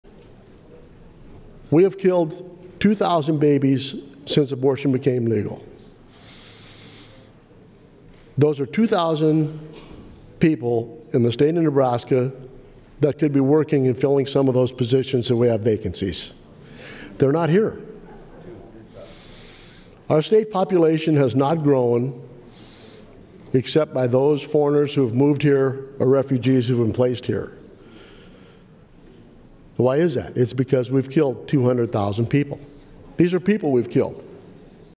NE State Sen. Erdman (R) points to the "Great Replacement" while arguing for an abortion ban
NE State Sen. Steve Erdman (R) uses Great Replacement talking points arguing for a six-week abortion ban.

"[We have] not grown except those foreigners who have moved here or refugees who have been placed here … because we've killed 200,000 people. These are people we killed."